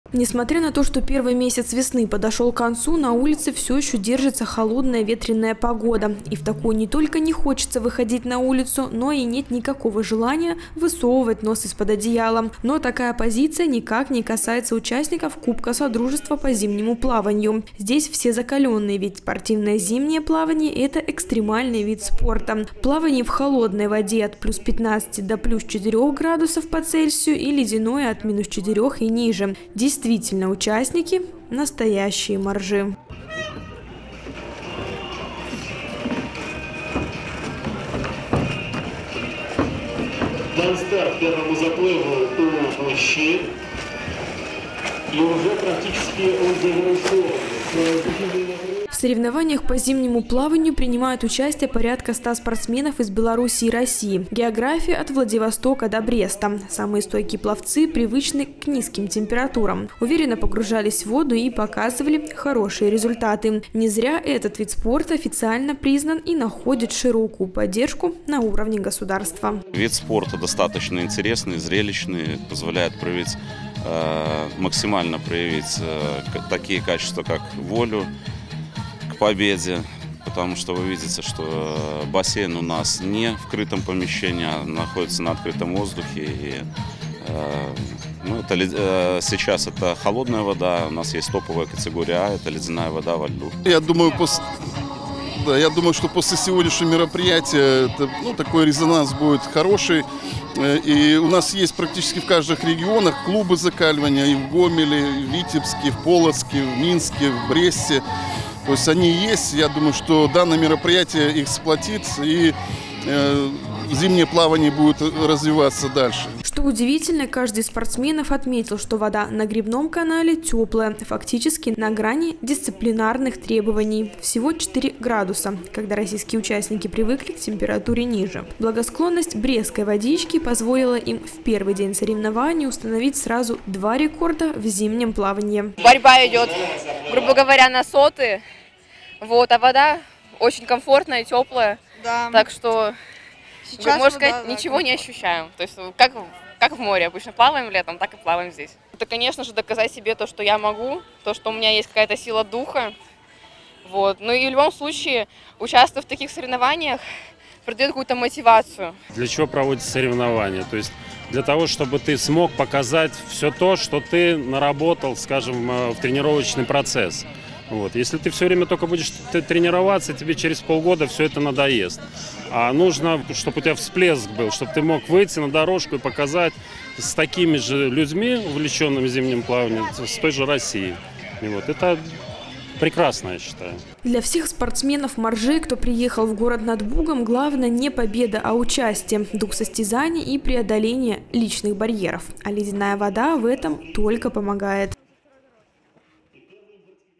2023-03-31 г. Брест. Международный кубок содружества по зимнему плаванию. Новости на Буг-ТВ.
Международный кубок содружества по зимнему плаванию «Брест — 2023».
Один из самых знаковых спортивных объектов Беларуси – Брестский Гребной канал – стал базой для проведения этапа Кубка мира по зимнему плаванию.  Из разных регионов Беларуси и России съехались пловцы-моржи, чтобы побороться за звание сильнейшего на различных дистанциях и стилях, установить новые рекорды и просто насладиться любимым делом. О том, как проходил Кубок содружества расскажет наш корреспондент Анастасия Мелишкевич.